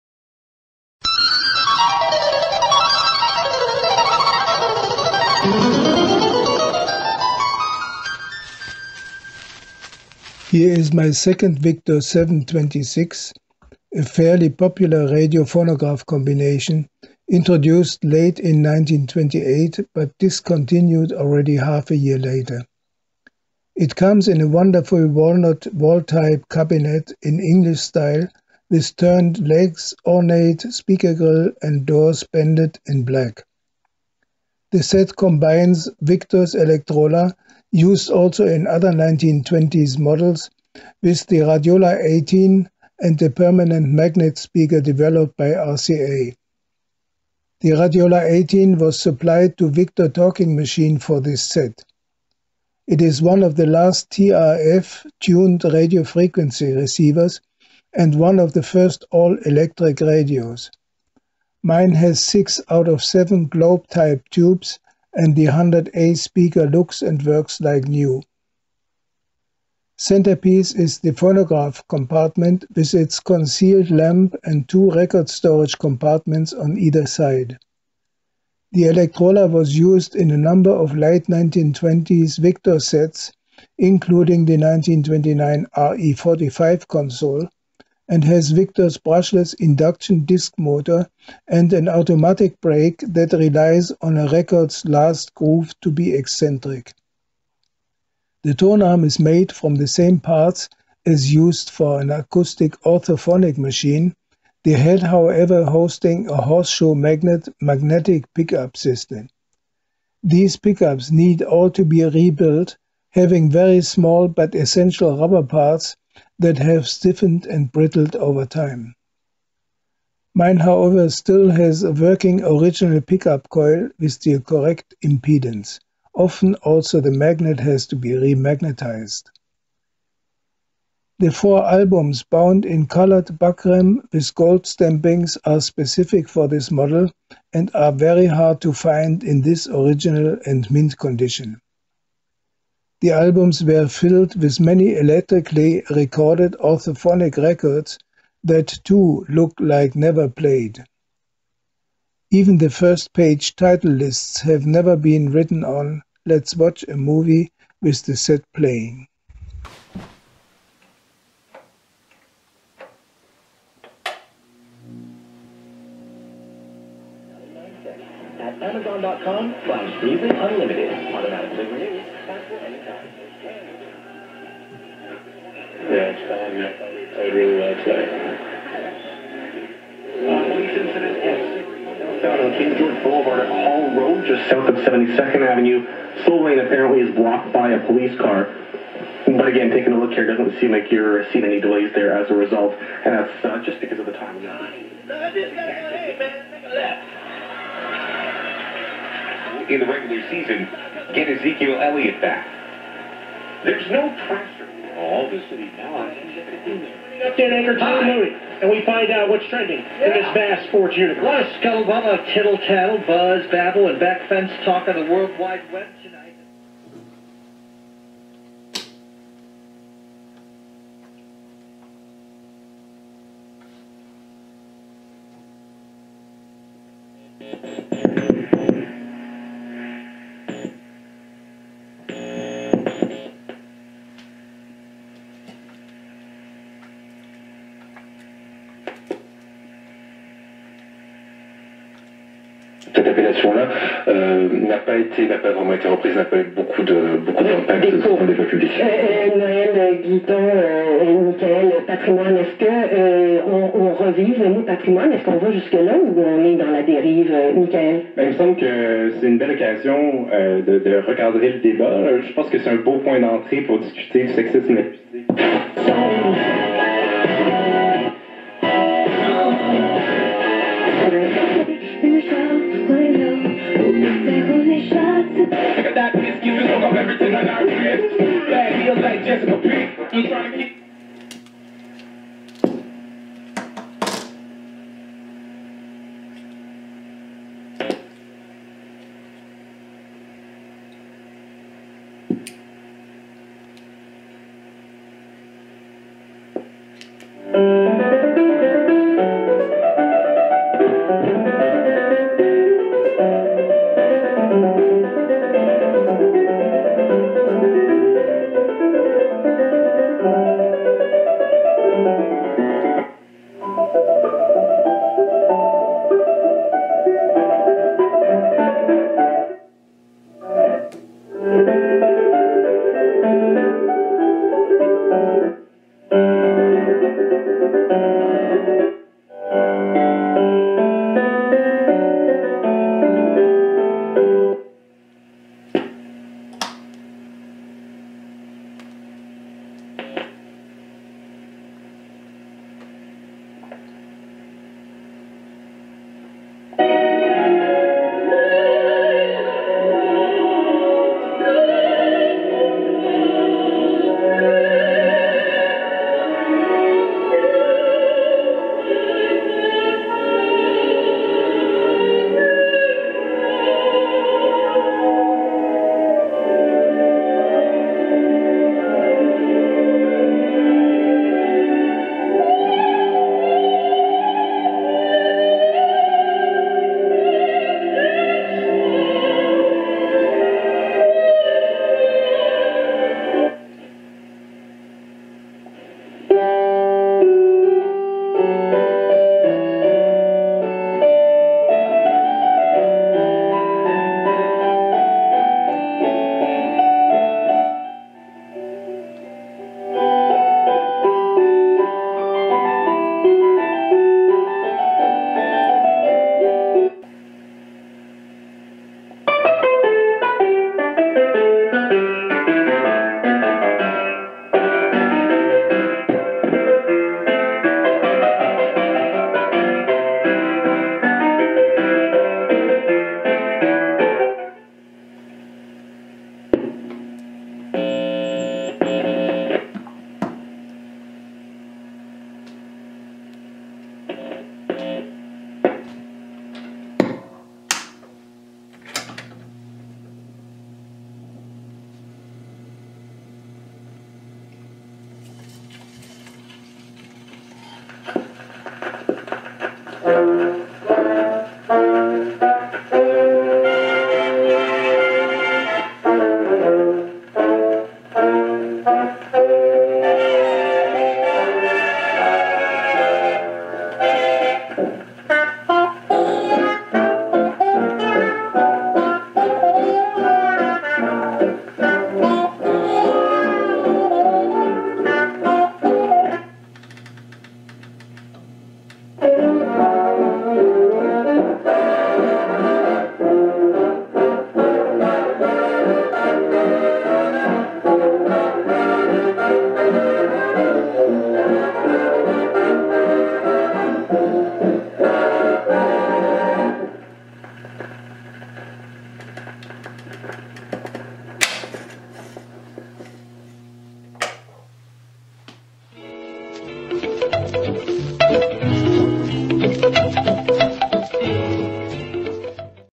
1928 Victor Victrola Electrola VE7-26X Serial 4336 Playing AM, mp3 and Vogue picture disc
After my first one here is my second Victor 7-26, a fairly popular radio/phonograph combination, introduced late in 1928 but discontinued already half a year later. It comes in a wonderful walnut wall type cabinet in English style with turned legs, ornate speaker grille and doors banded in black. The set combines Victor's Electrola, used also in other 1920's models, with the Radiola 18 and a permanent magnet speaker developed by RCA. The Radiola 18 was supplied to Victor Talking Machine for this set; it is one of the last TRF Tuned Radio-Frequency receivers and one of the first all-electric radios. Mine has 6 out of 7 globe type tubes and the 100A speaker looks and works like new. Centerpiece is the phonograph compartment with its concealed lamp and two record storage compartments on either side. The Electrola was used in a number of late 1920's Victor sets including the 1929 RE-45 console and has Victor's brushless induction disc motor and an automatic brake, that relies on a records last groove to be eccentric. The tonearm is made from the same parts as used for an acoustic orthophonic machine, the head however hosting a horseshoe magnet magnetic pickup system. These pickups need all to be rebuilt, having very small but essential rubber parts that have stiffened and brittled over time. Mine however still has a working original pickup coil with the correct impedance. Often also the magnet has to be re-magnetized. The 4 albums bound in colored buckram with gold stampings are specific for this model, and are very hard to find in this original and mint condition. The albums are filled with many electrically recorded orthophonic records, that too look like never played. Even the first page title lists have never been written on. The set was for sale at Radio Antiks (ref.1).